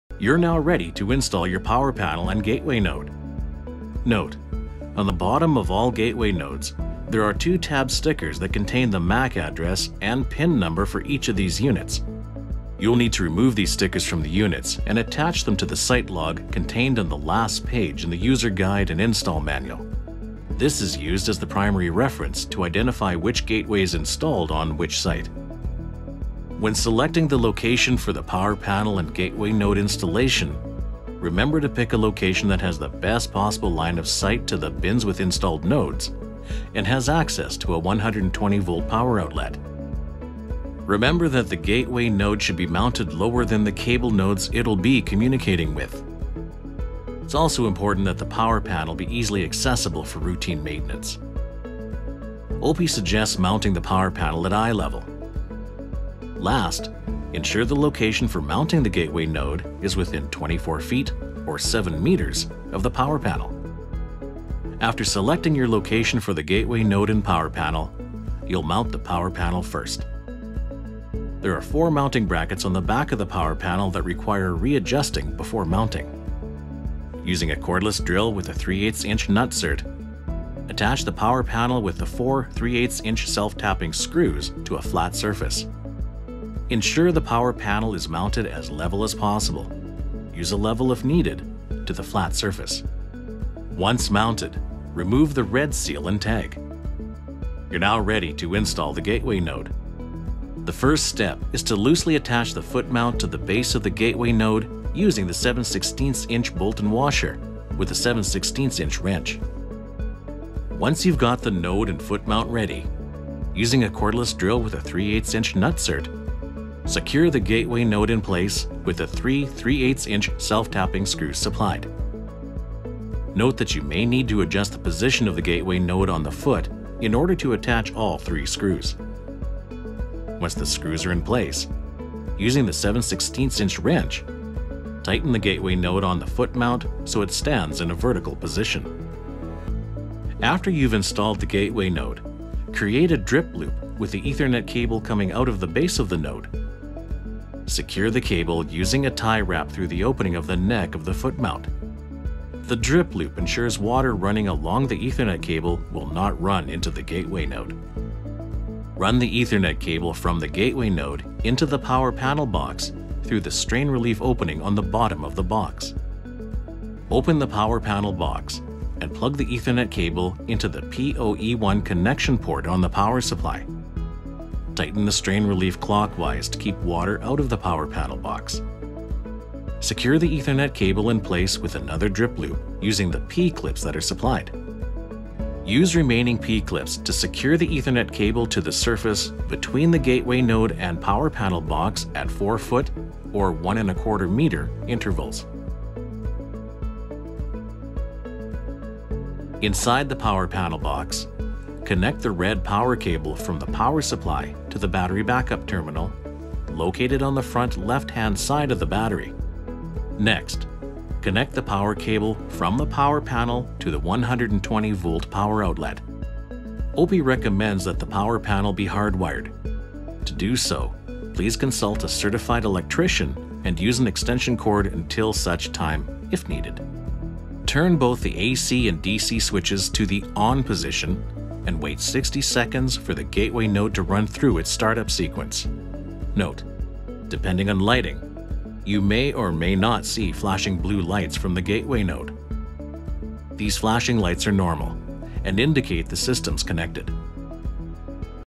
OPI Blue Installation Video - Gateway Installation & Electrical Connections
How to install a Gateway Node and Power Panel for an OPI Blue grain management system.